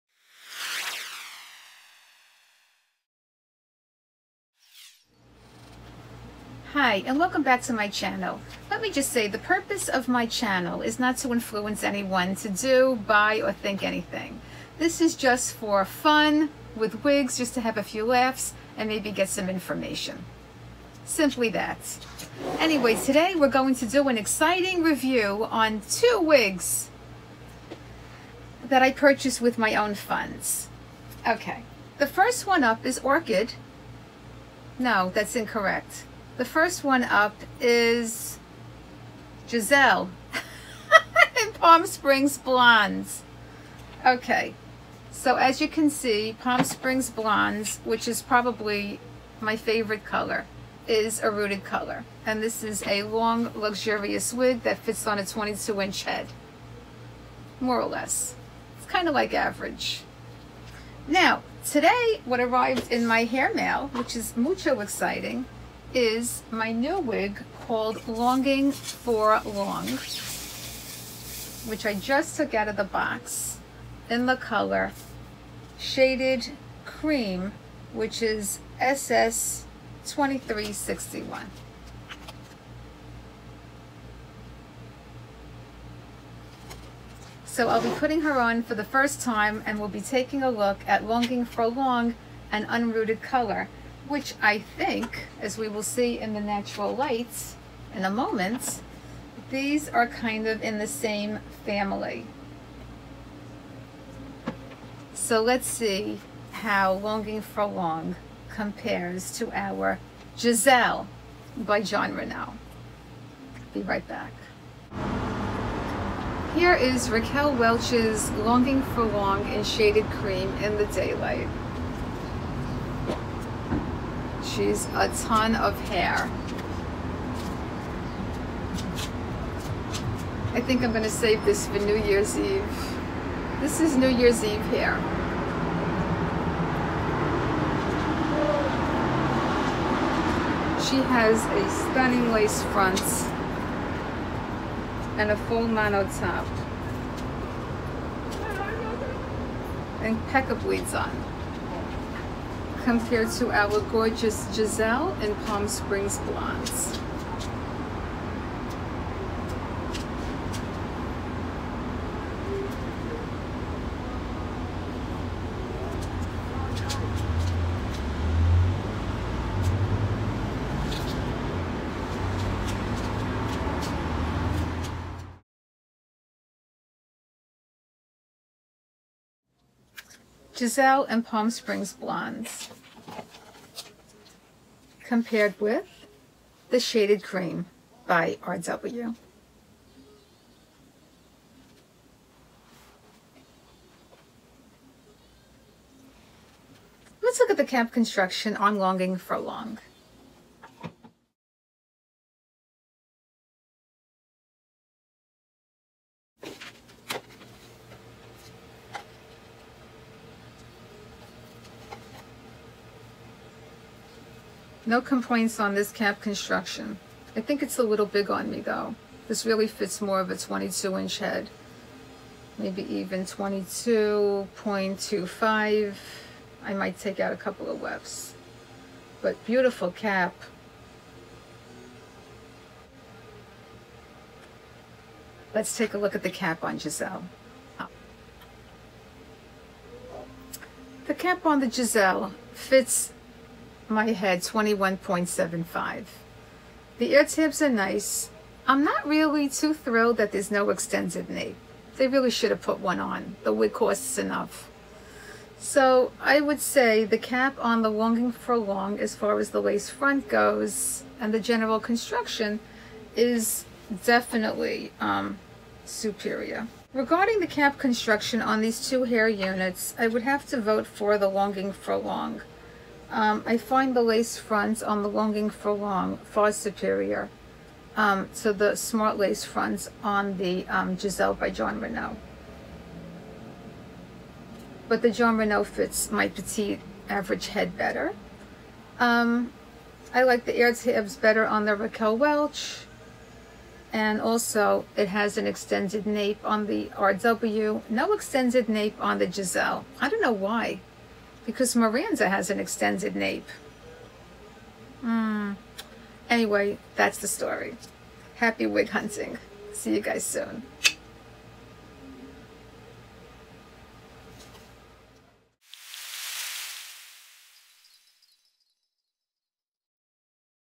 Longing For Long/Gisele
Longing for Long by Raquel Welch in the color SS Cream SS 23/61 (A Shadow Shade) compared to Gisele by Jon Renau in Palm Springs Blonde FS 17/101S18 (A Rooted Shaded). Both units are Synthetic NOT Heat Friendly. Note: Longing for Long fits a 22 inch head to 22.50 whereas Gisele fits 21.25 to 22 inch head in my opinion. Hair units purchased by me. No ads.